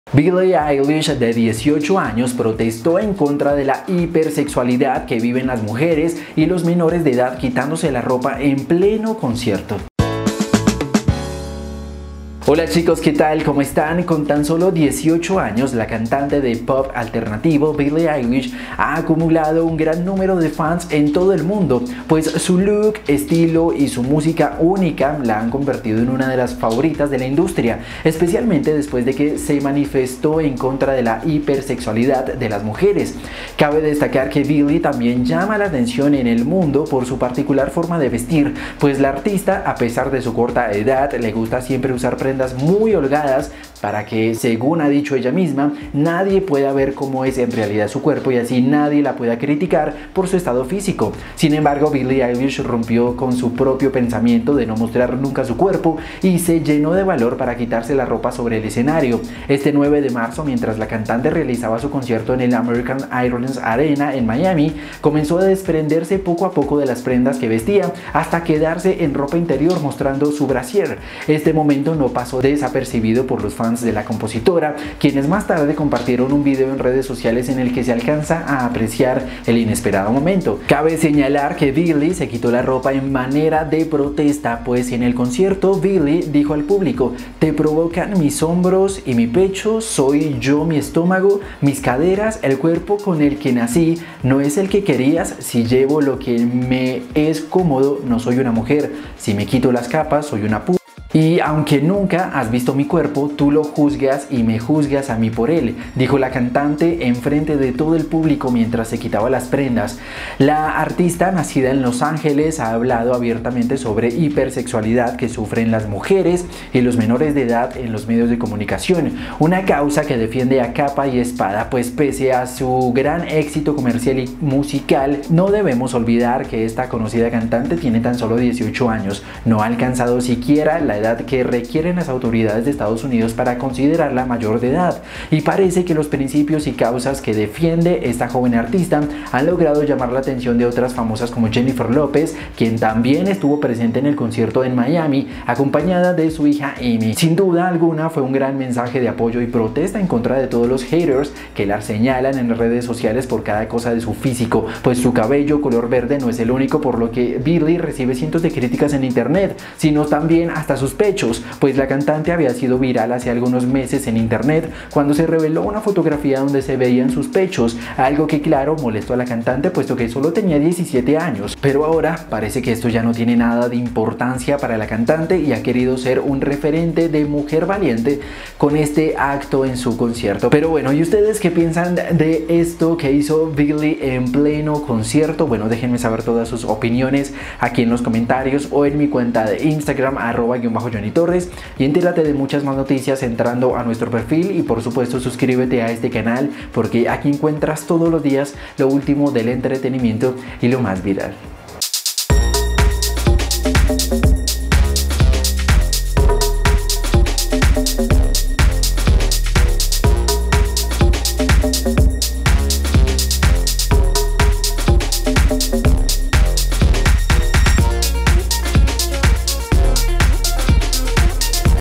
Billie Eilish se quita la ropa en pleno concierto y deja ver su cuerpo como respuesta a sus haters
Todas las imágenes y videos presentados son tomados para uso periodístico y con el único fin de informar a nuestra audiencia. Speak News es una propiedad de Speak Entertaiment